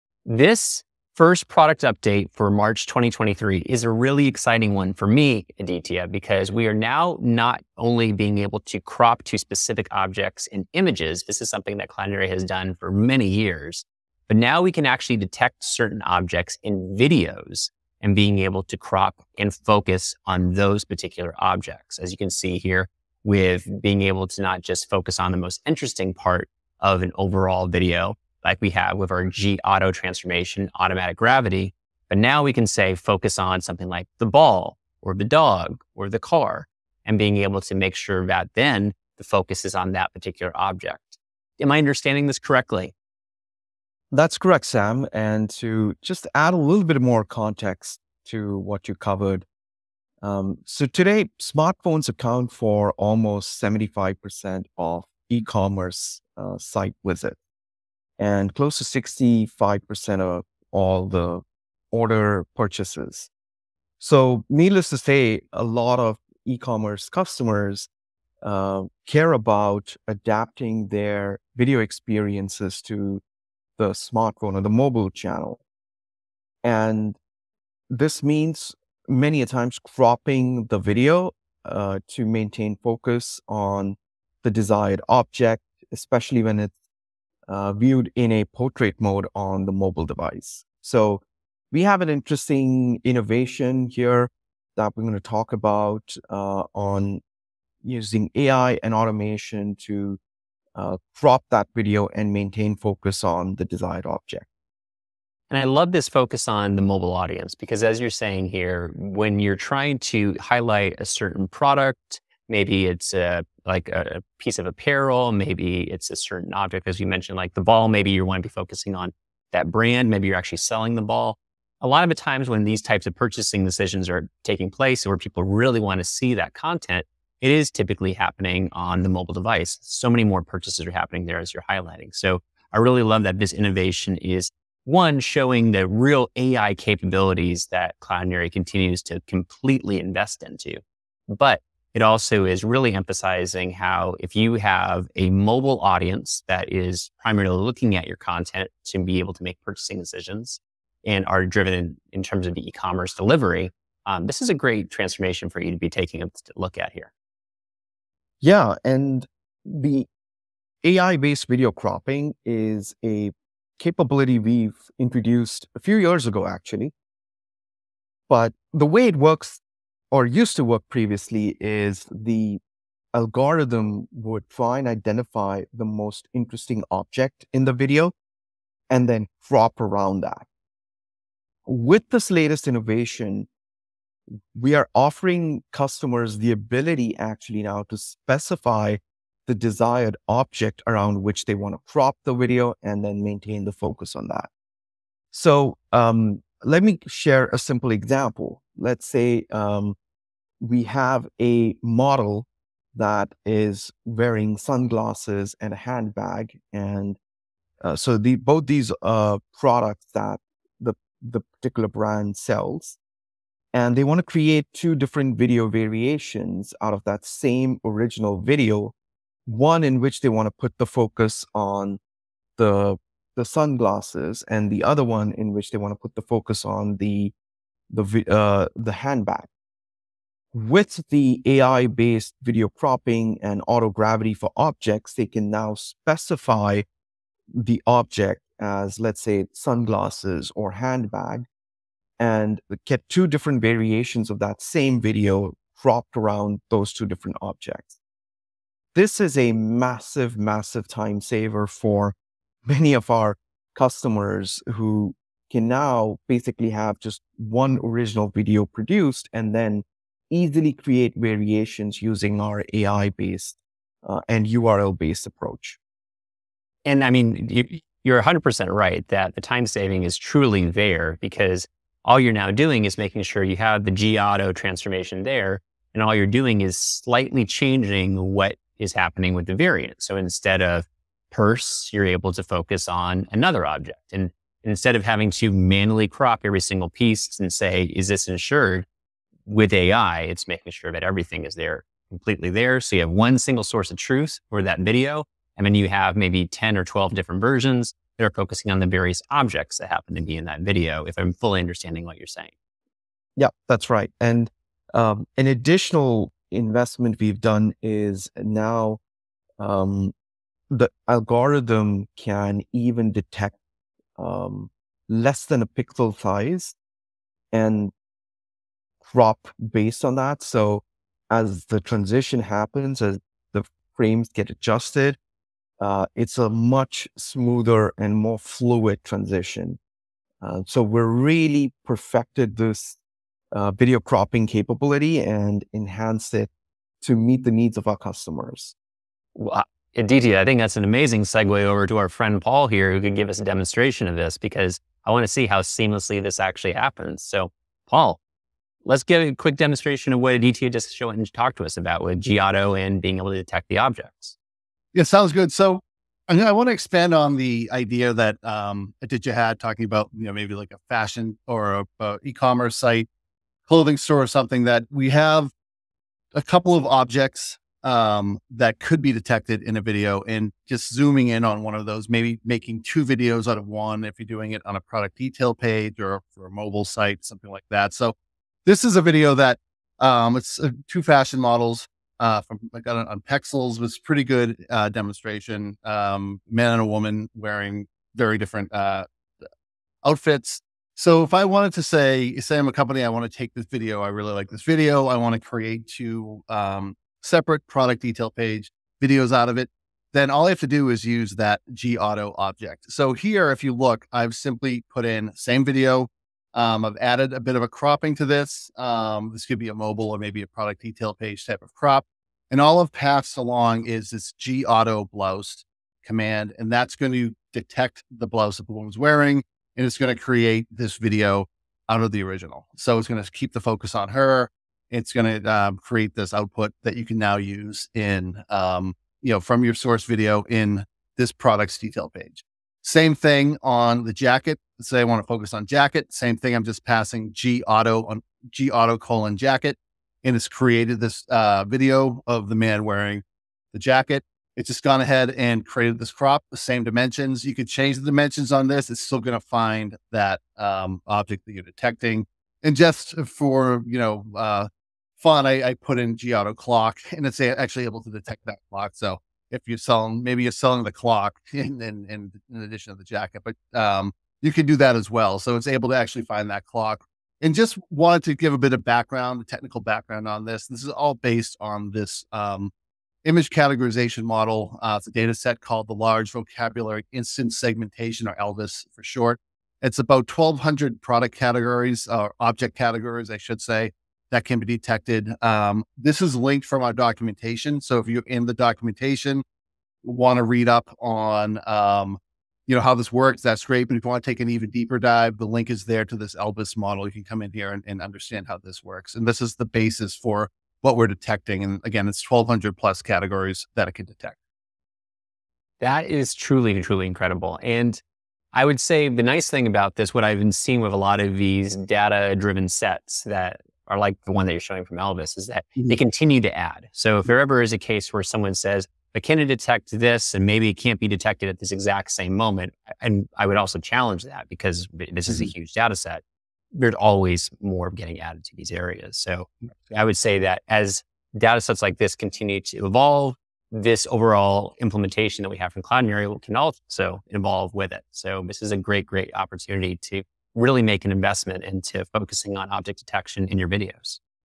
AI Video Cropping: Auto-Gravity for Objects - Cloudinary Product Update
Cloudinary's auto-gravity for videos uses AI to automatically detect and crop the most relevant part of a video. This can be particularly useful for adapting existing videos for viewing on a smartphone by cropping a portrait sized section from a landscape aspect video.

The AI auto-gravity for videos can now be adjusted so that instead of just focusing on what the algorithm determines is the most interesting areas of the video, it can focus on a specific object, with g_auto: object. It then uses this information to automatically crop the video to focus on the most relevant content in the frame.

You can learn about all of Cloudinary's Programmable Media updates from March 2023 in our 25-minute, self-paced workshop

More information about Cloudinary's video cropping and resizing features are available in our documentation

Not a Cloudinary user yet?